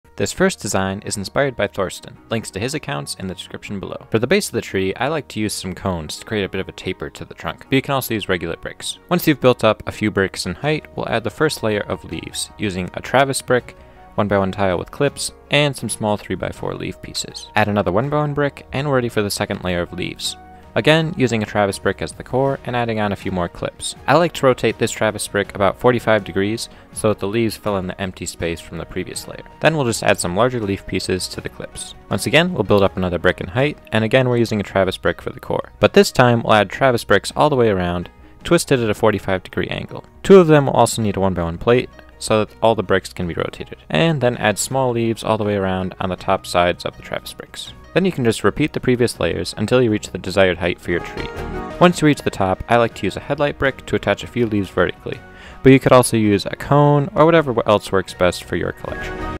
Realistic LEGO Pine Tree Design | Easy Tutorial
Looking for a simple and sturdy pine tree design to use in one of your LEGO builds? In this tutorial we cover a simple and sturdy technique using just a few parts.